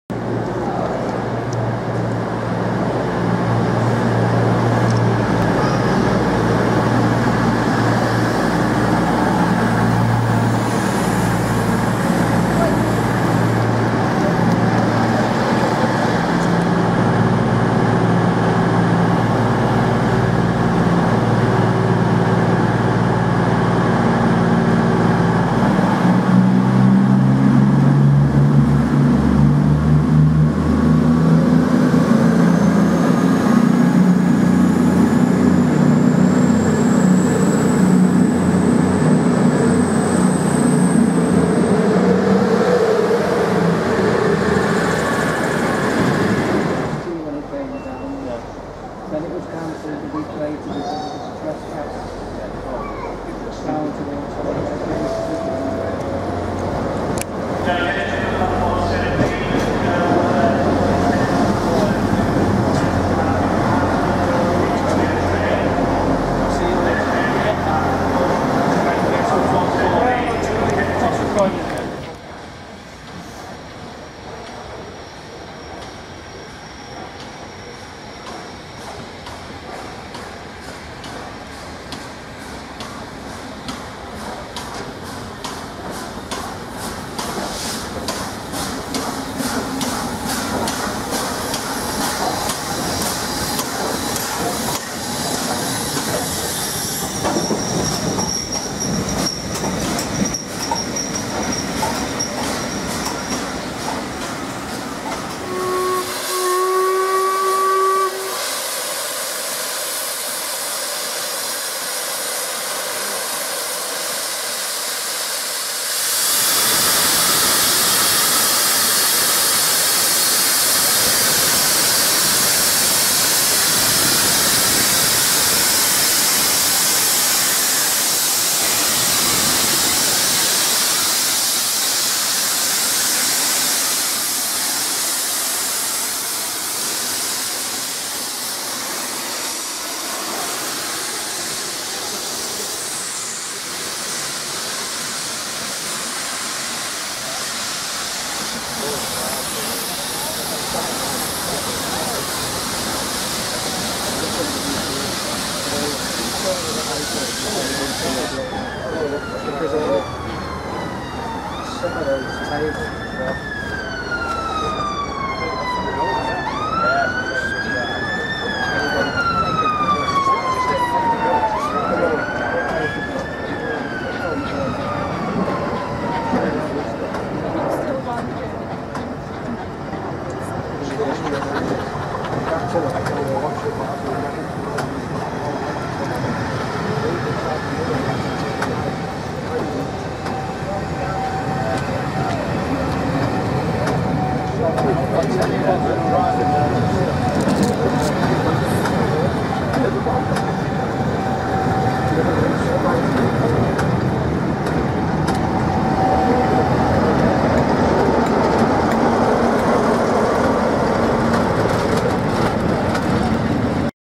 Trainspotting at Chester 14-4-18
Didn't do much trainspotting at Chester this time as I was only there for around 2 hours. The main event was to see 46100 "Royal Scot" which arrived in Chester just before 10:30 this morning. Hope you all enjoy

Music Used in this video is courtesy of the artist.